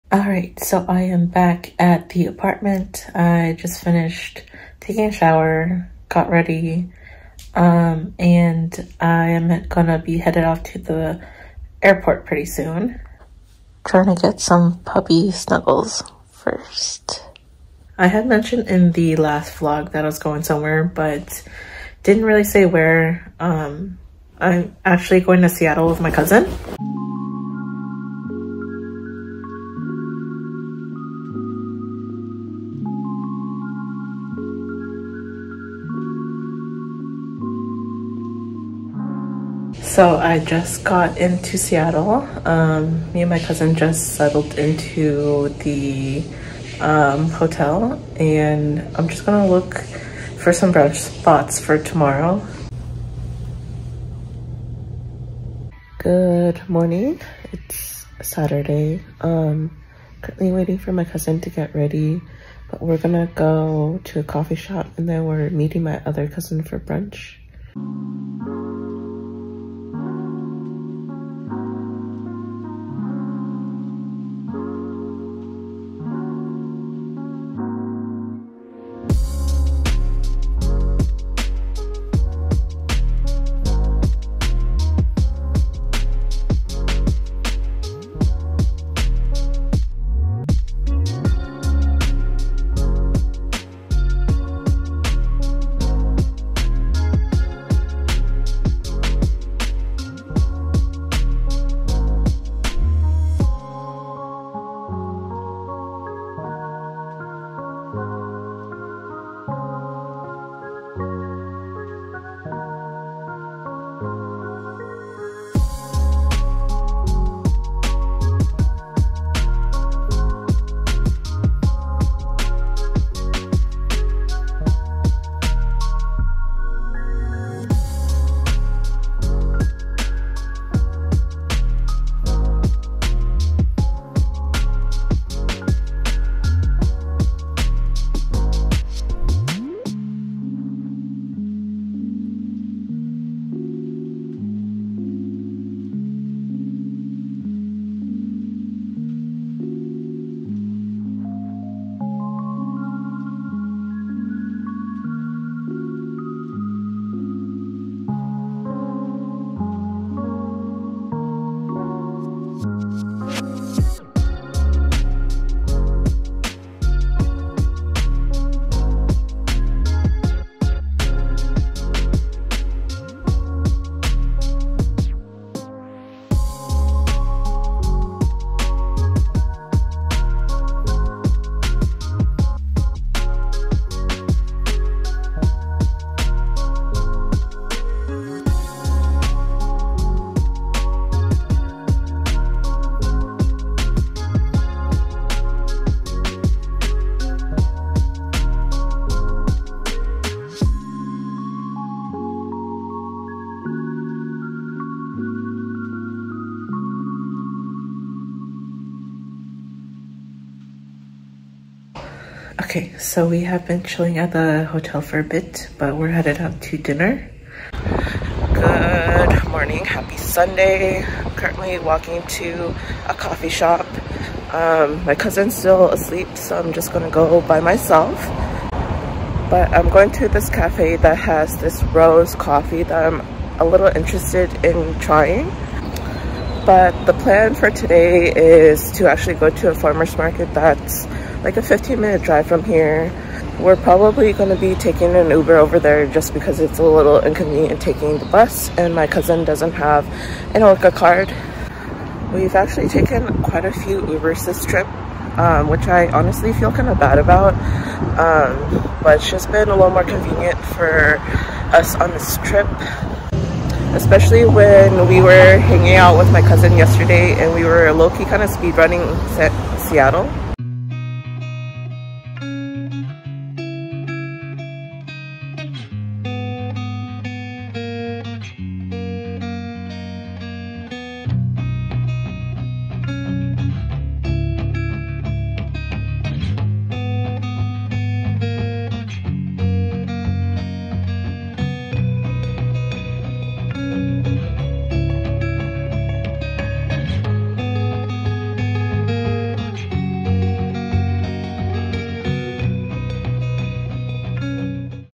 Seattle Weekend Trip | May 3rd - 5th, 2024
Hey y'all!
My cousin and I went on a quick romp over to Seattle for the weekend! We got to visit some of the big touristy places in Seattle and also hang out with our cousin who lives in Seattle!